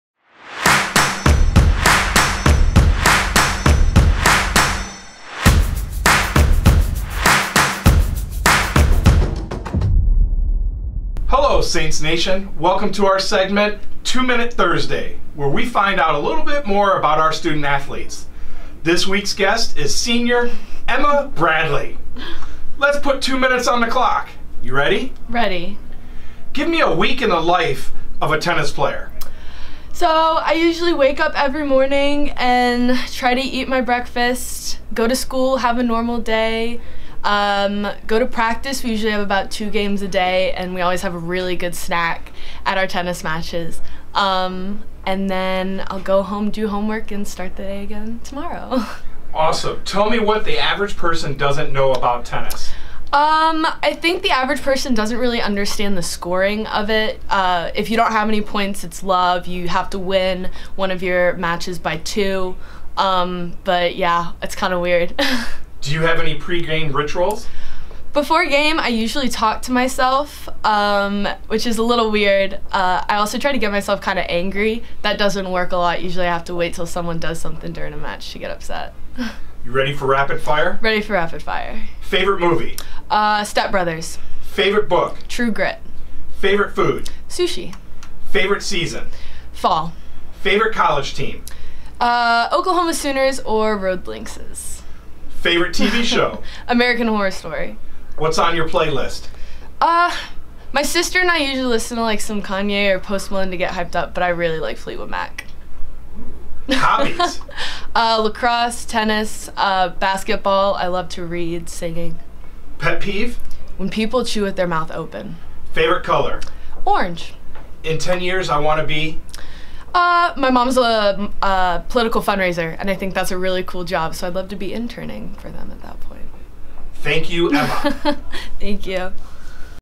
Two Minute Thursday! With Emma Bradley - Tennis
Our new weekly segment - Two Minute Thursday - Where we get to know a little more about the student-athlete!  Come watch!